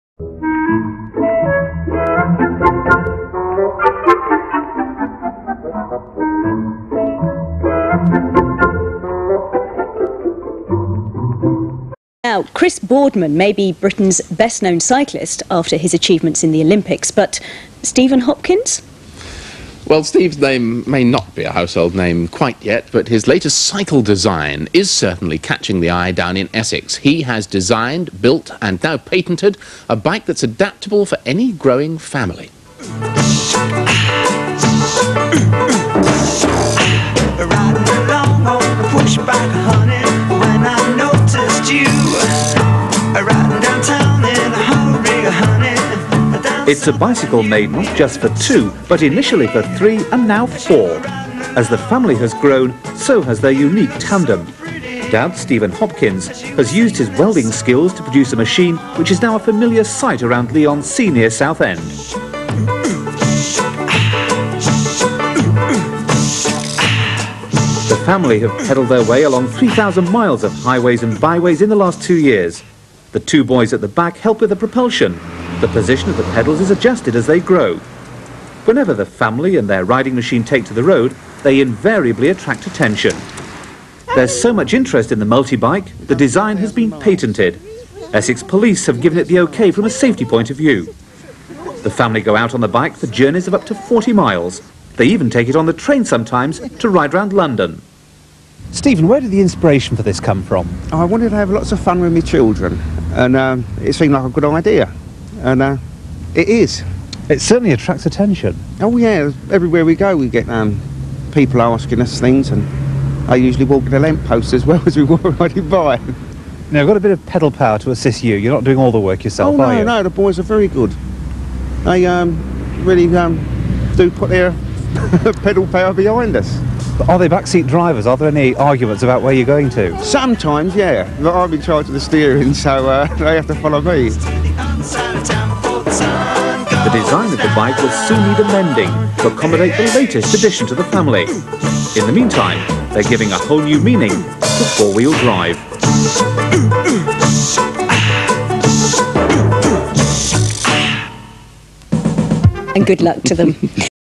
Hopkins Family Cycle
This multi person bicycle was designed and built by an engineer from Leigh-on-Sea, Essex, England in 1990. The bike, and a couple of others built by S Hopkins, were seen around Essex from the late 1980s to the 1990s. Much interest was shown by bicycle retailers but the Hopkins Family Cycle never went into commercial production due largely to insufficient investment capital. Although an historical testament to the inventive genius of one parent motivated primarily to raising healthy children, it is a pity that such an inexpensive, healthy and environmentally friendly means of family transport did not become widespread. Instead we have entered the 21st century with a glut of weak and obese children driven around in motor vehicles by coach-potato parents.